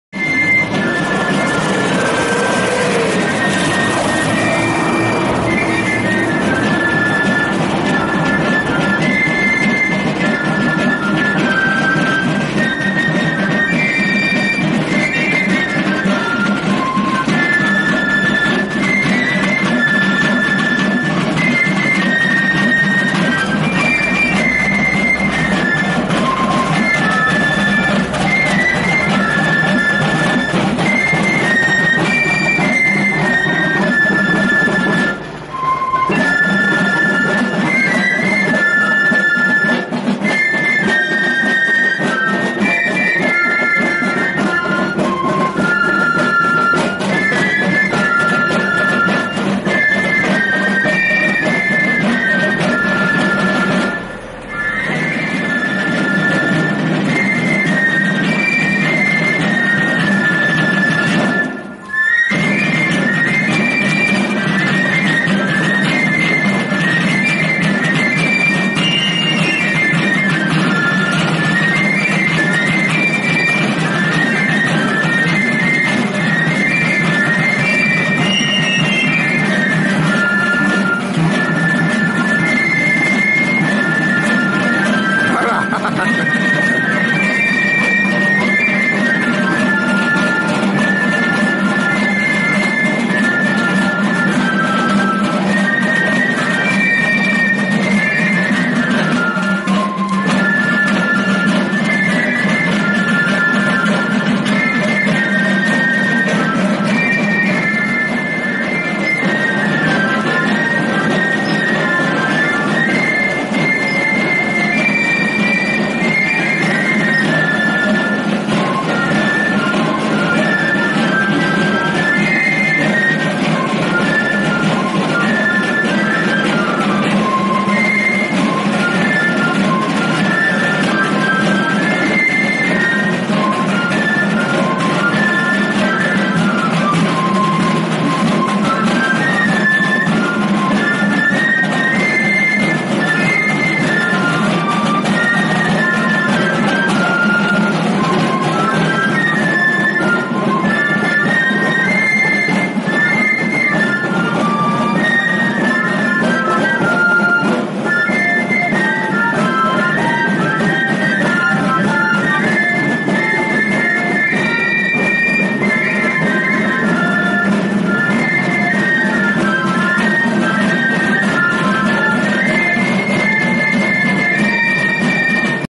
2019ko Durangaldeko Txistularien Eguna Kalejira
2019ko martxoaren 23a Elorrio
Irudiak. Juan Antonio Aroma
Gehiago jakiteko: